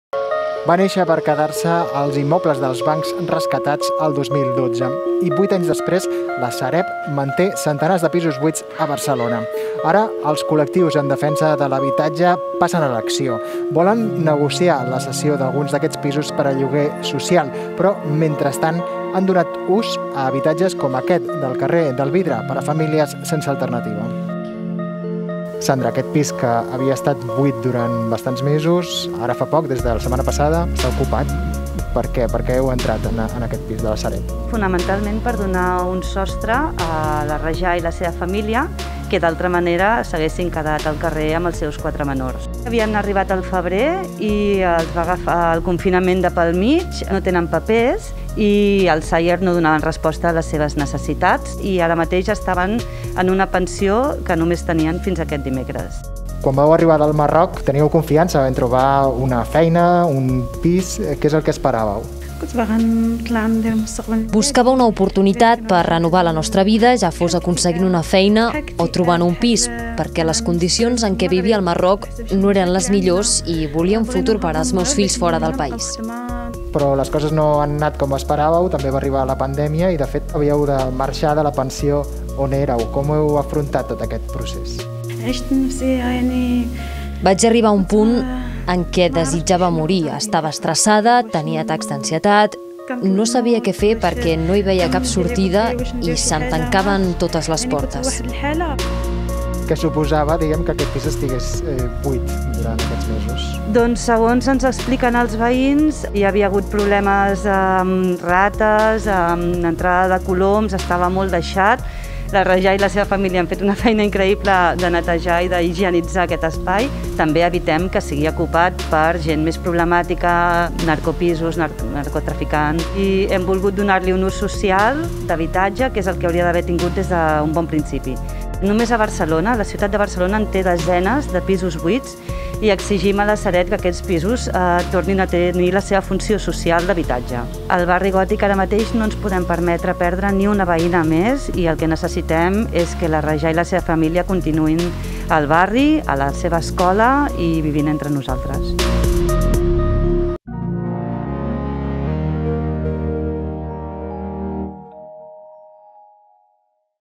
Ocupen un pis buit de la Sareb per exigir-li lloguers socials
Estava buit des de feia mesos. Hi havia rates, hi entraven coloms i estava “molt deixat”, segons els veïns. La situació, però, ha canviat en pocs dies en aquest tercer pis del carrer del Vidre, propietat de la Sareb, coneguda com a “banc dolent”. Resistim al Gòtic ha anunciat aquesta setmana “l’alliberament” de l’habitatge per donar-hi ús i proporcionar de pas un allotjament a una família del barri “sense alternativa d’habitatge”.

Rajaa Taidi, el marit i els quatre fills menors ocupen ara el pis. S’hi han mudat després d’un periple que ha estat a punt de deixar-los al carrer. Van venir del Marroc al febrer “buscant una oportunitat”, però els va enganxar la pandèmia. Ara vivien en una pensió però n’avien de marxar aquesta mateixa setmana. Sense papers i sense recursos, els Serveis Socials els van adreçar al SAIER (el Servei d’Atenció a Immigrants), “però no donava resposta a les seves necessitats d’habitatge”, segons la portaveu de Resistim al Gòtic, Sandra Caballero.